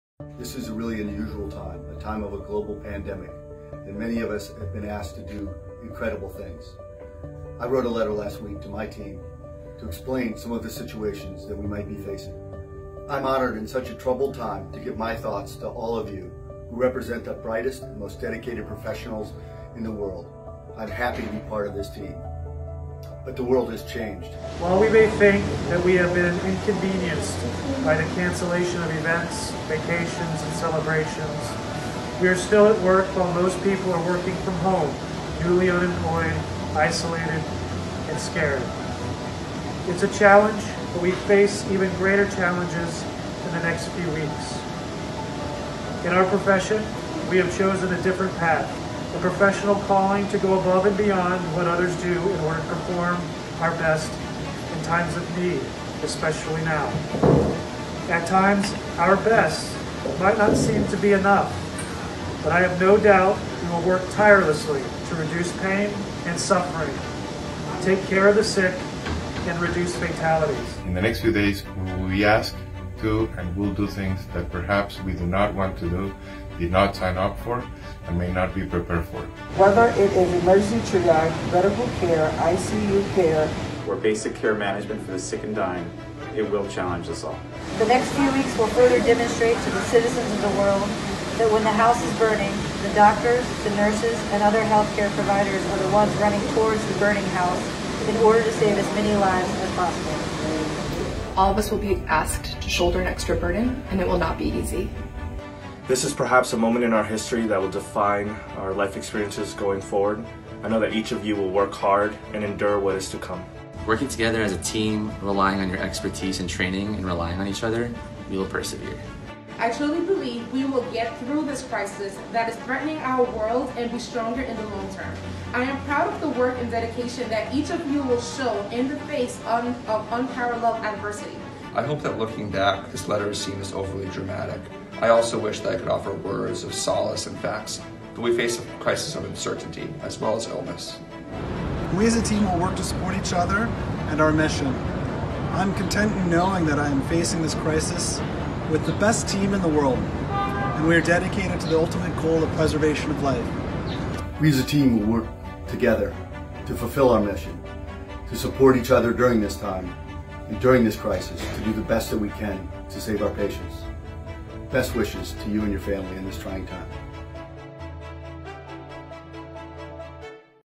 Healthcare Heroes Find Hope in Letter from Montefiore-Einstein’s Dr. Scott Melvin
Montefiore heroes read aloud an inspirational letter from their boss Dr. Scott Melvin, Vice Chair of Clinical Surgery. Filled with words of wisdom and hope, this motivational rally is defined by perseverance and bravery, during a moment in time we'll never forget.